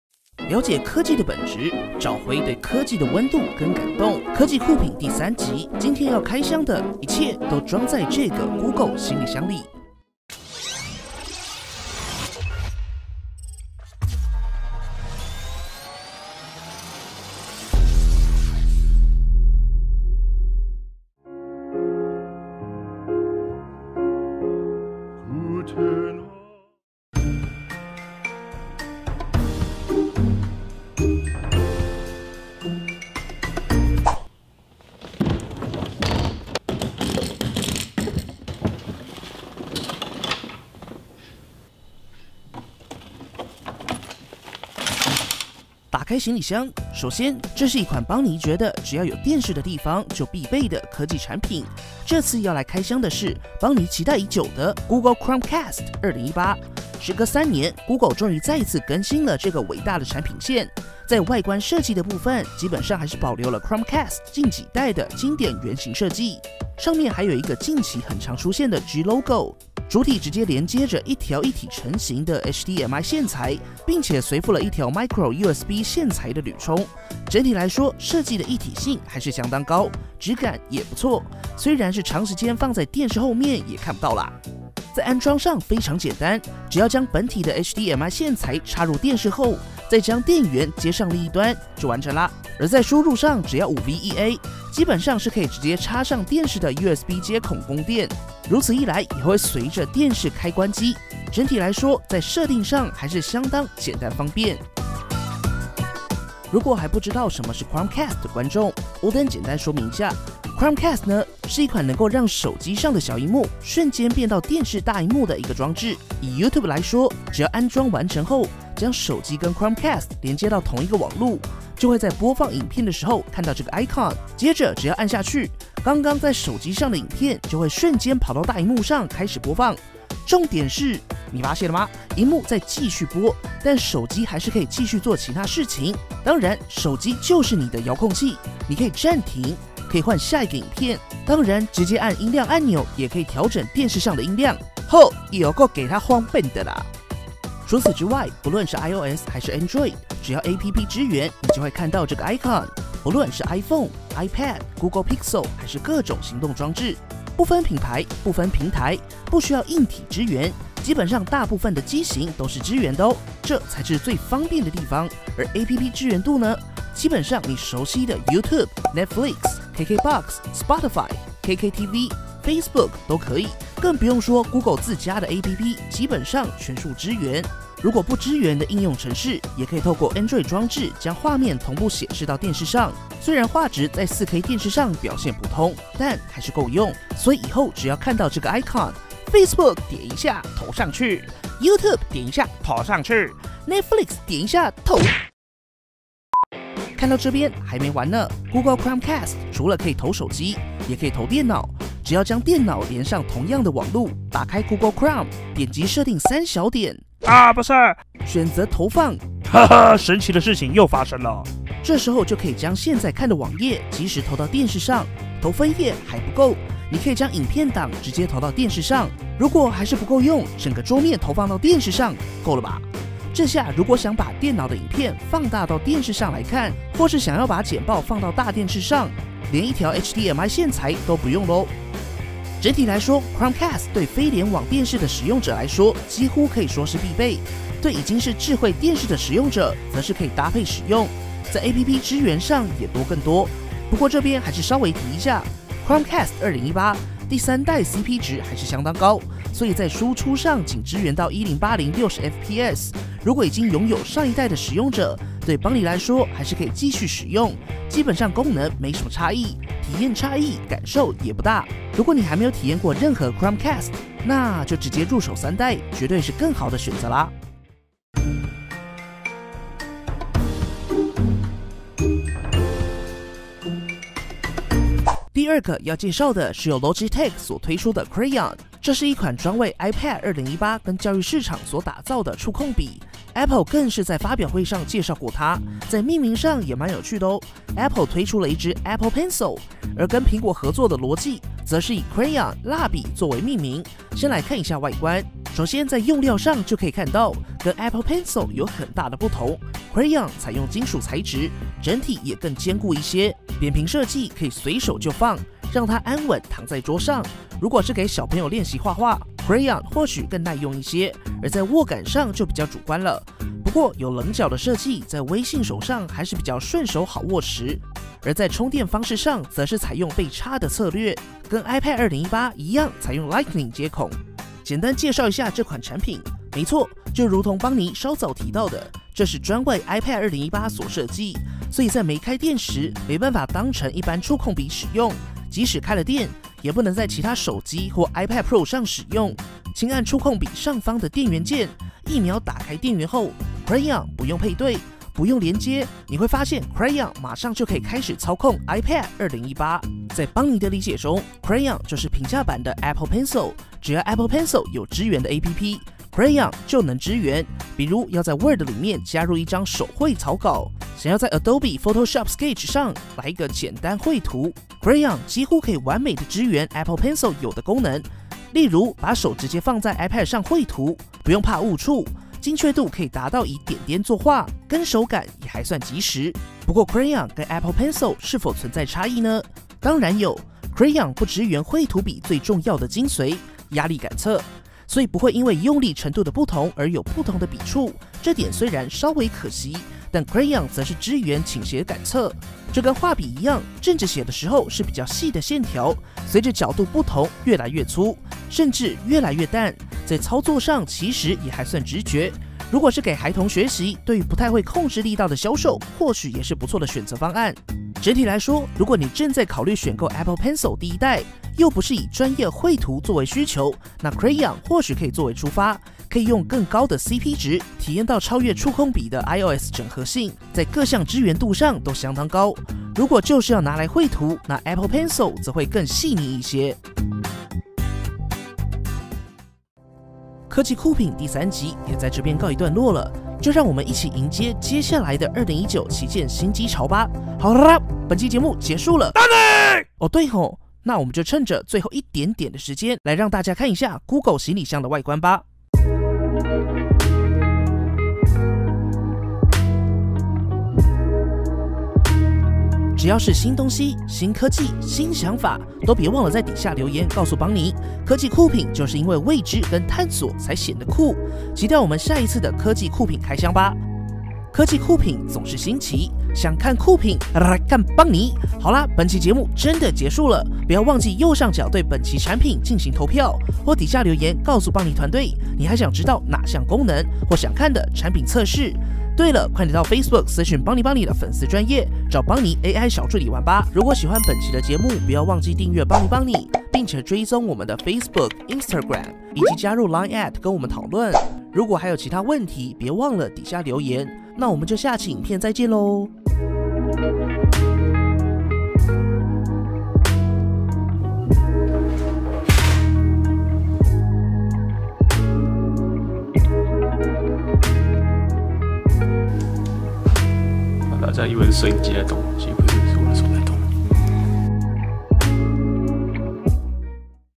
Evaluation and technology of cool products
a brand new out-of-the-box evaluation series. Bonnie will help you with the new cool series. Bonnie will take you to explore the novelty of technology, understand the essence of technology, and find the temperature and touch of technology. Through the real sharing of our experience, perhaps not comprehensive, but definitely cut everyone's pain points. The new technology products brought by this time include luggage from Google, Google Chromecast 2018 (third generation) out of the box evaluation, evaluation, recommendation, value is not worth buying. Through super-complete test comparison, in addition to telling you that the value is not worth